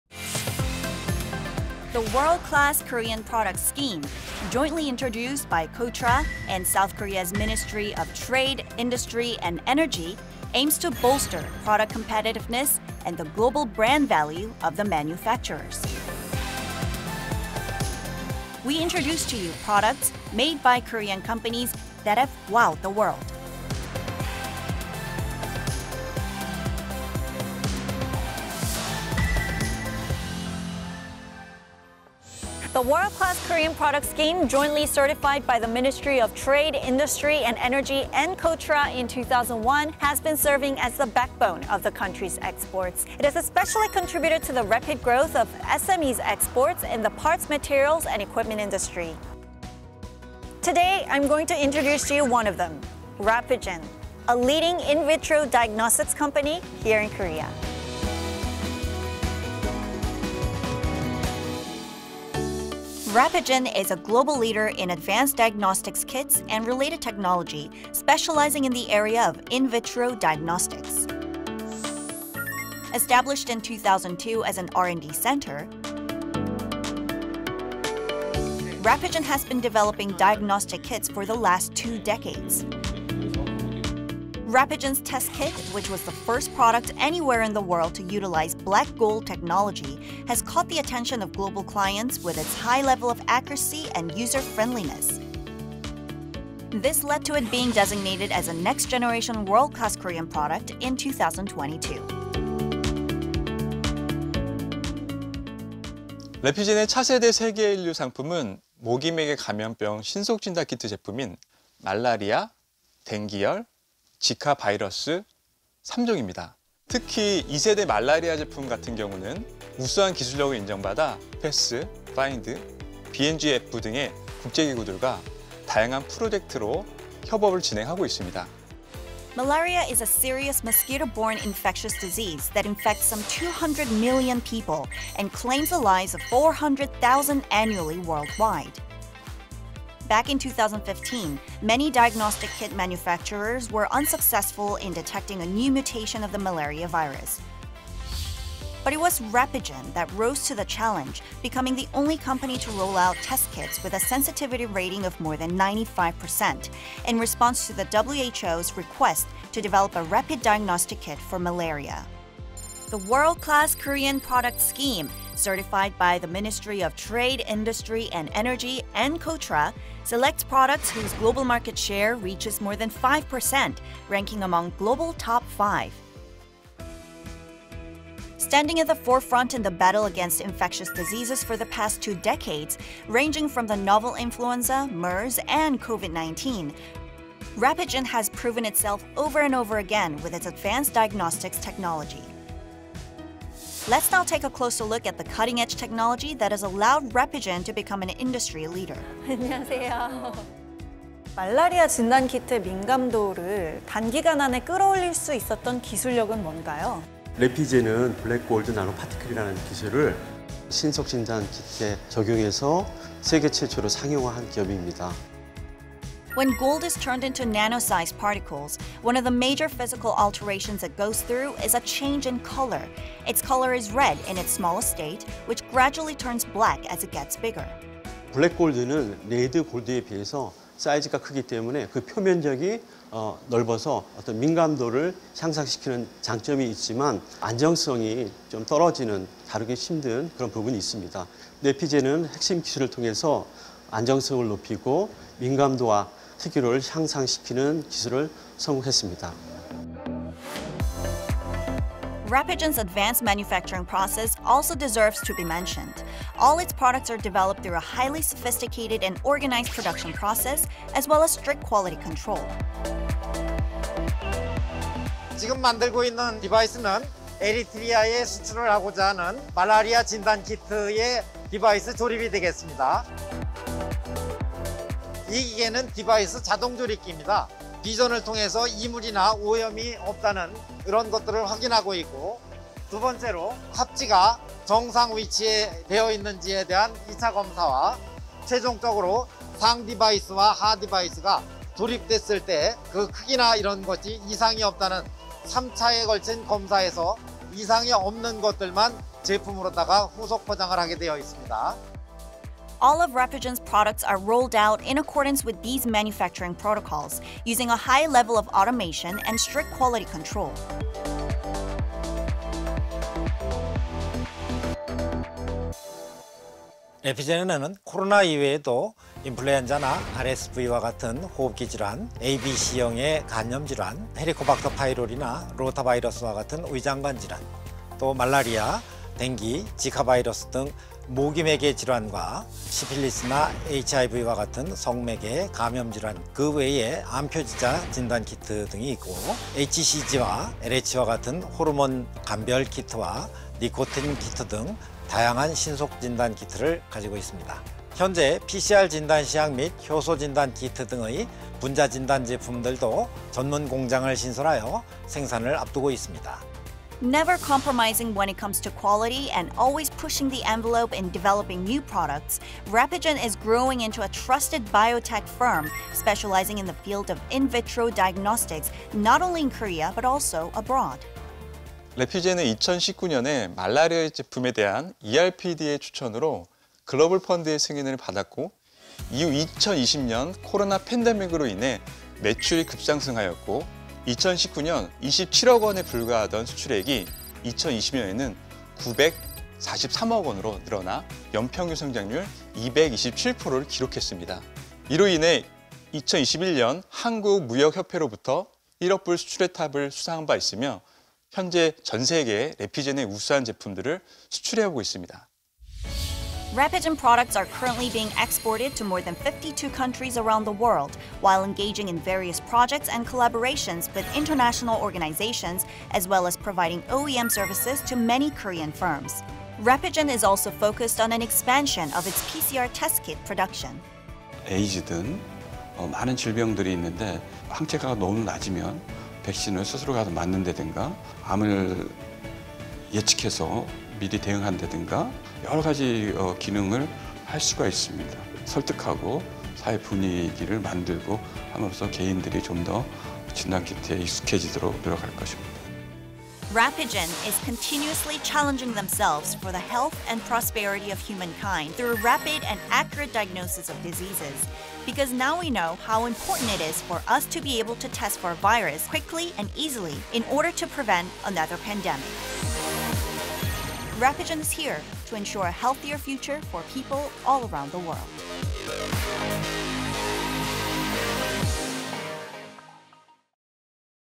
[PREMIUM KOREA] Rapigen, a pioneer in the in vitro test field and diagnosis technology possessing...
[PREMIUM KOREA] Rapigen, a pioneer in the in vitro test field and diagnosis technology possessing outstanding source technology

Rapigen, a pioneer in the in vitro test field and diagnosis technology possessing outstanding source technology, now stands at the center of global attention by becoming the first player to commercialize Black Gold Particle Technology in rapid test kits. In this episode, we introduce to you the rapid test kits for 3 major mosquito-borne - Malaria, the Dengue virus, and Zika virus - which has been designated as Next-generation World Class Product of Korea in 2022.

체외진단키트 및 진단기술 분야에 선도적이며 우수한 원천기술을 보유한 체외진단 전문기업 레피젠은 신속진단키트 분야에 블랙 골드 기술 최초 상용화해 주목을 받고 있다. 이번 편에서는 2022년 차세대 세계일류상품으로 선정된 모기 매개 감염병 신속진단키트 제품인 말라리아, 뎅기열, 지카 바이러스 3종을 소개한다.